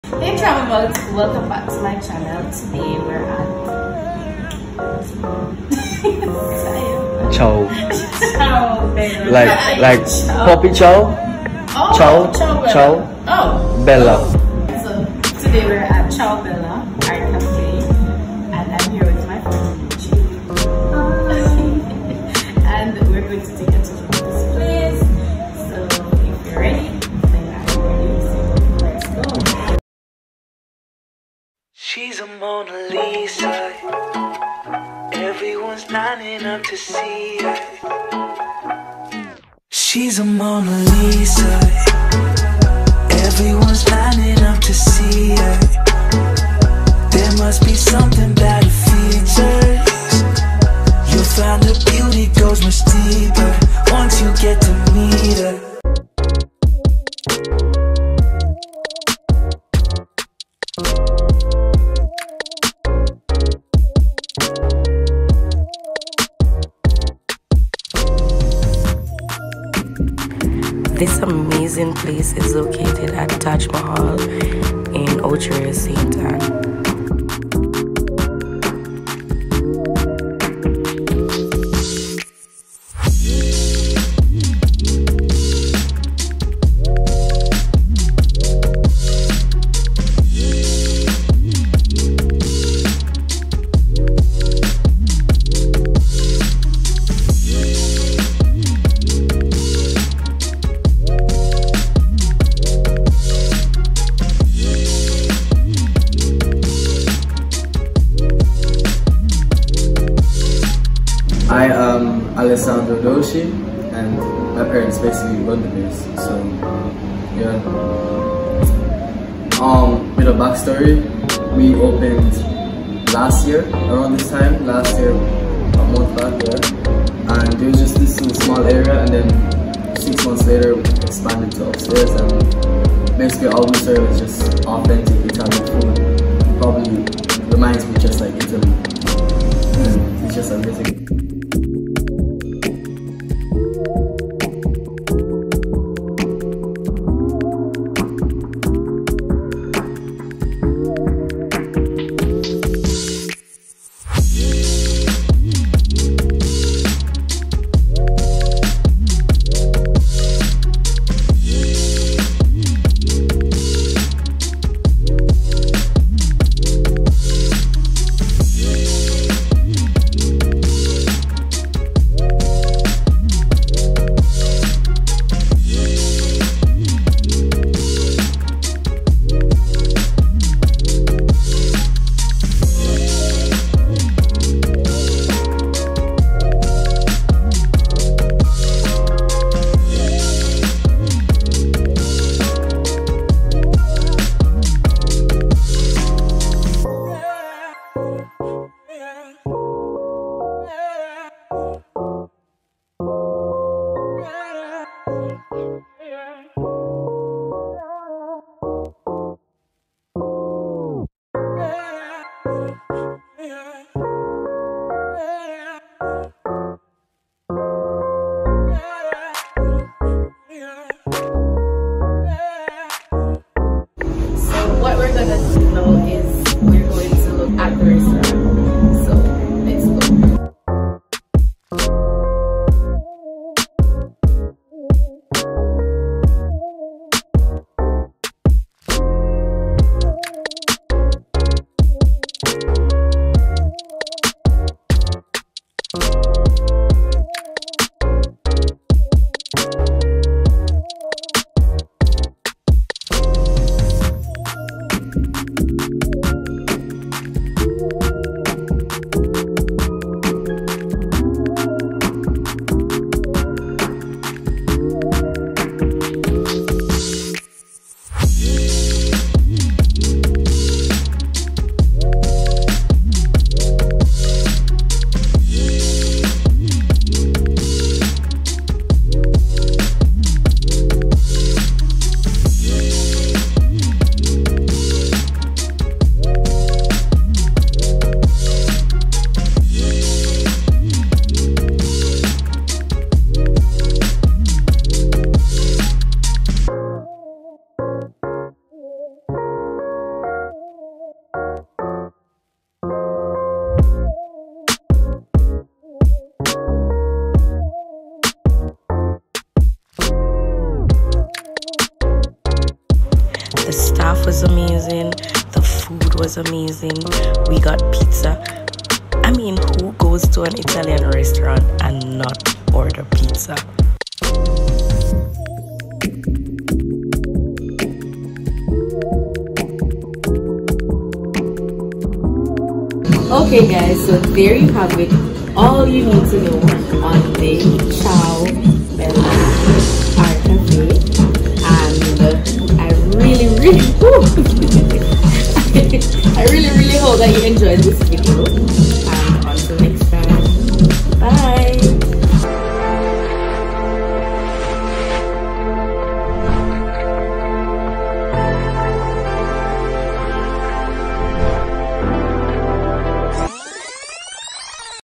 CIAO BELLA ART CAFÉ|| TAJMAHAL, OCHO RIOS, ST. ANN, JAMAICA🇯🇲
🌻👇🏽ABOUT👇🏽🌻

Principals Avi Doshi and Tiziana Censi converted space within their 876 Butik store, located inside the Taj Mahal Plaza in Ocho Rios, for what is now one of the must try eateries decorating the food landscape in St Ann.
The menu consists of Italian street food favourites including supplì, panini, pasta, pizzette, lasagne and salads.The café also features an assortment of beers, wines and cocktails.
This is quite noteworthy as both principals are very accessible and always sharing titbits and interacting with customers.

🌻THANK YOU SOOOO MUCH FOR WATCHING! I APPRECIATE IT!!!